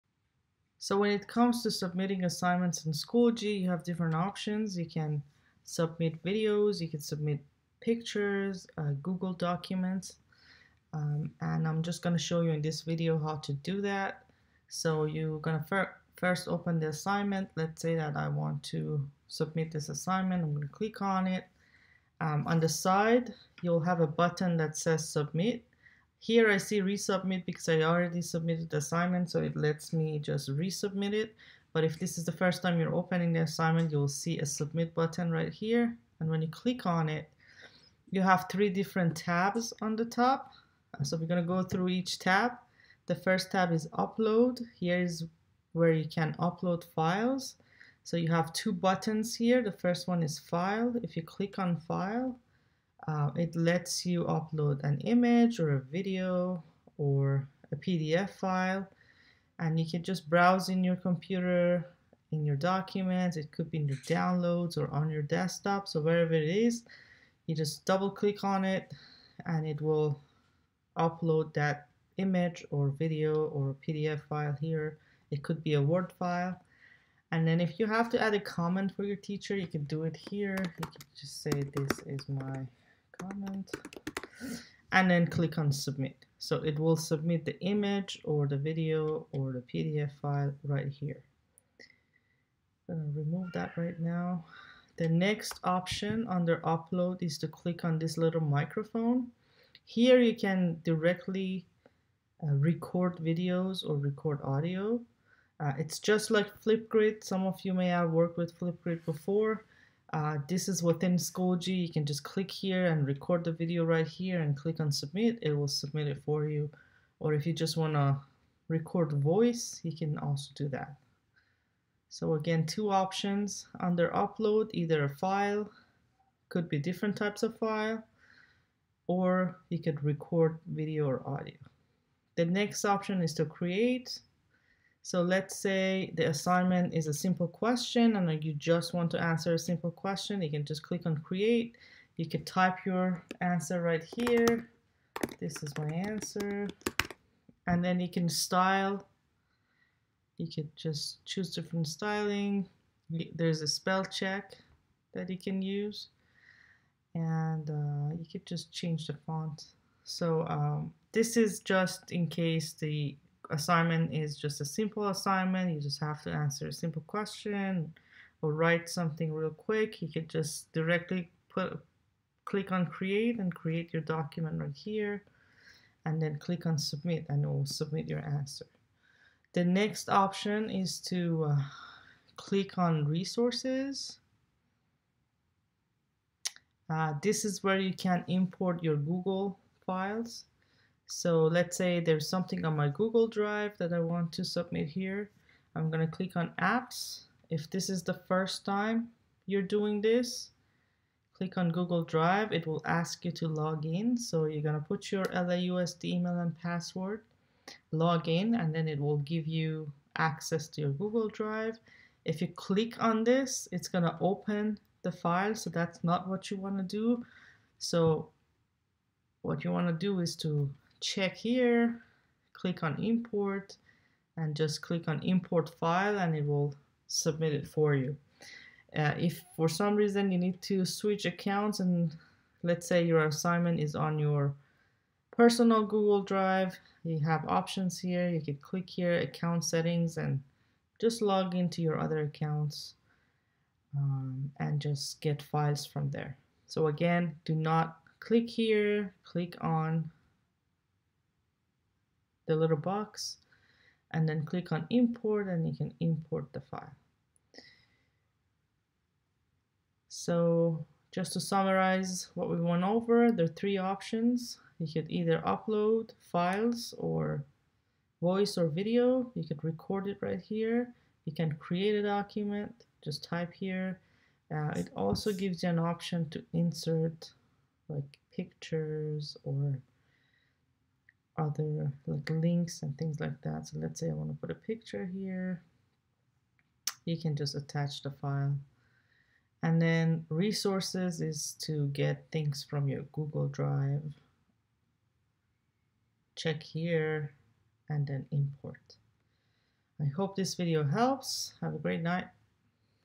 Schoology Tip - Submit Assignments (image, video, audio, Google docs...)
This video shows how to submit images, videos, audio files, and Google docs in Schoology.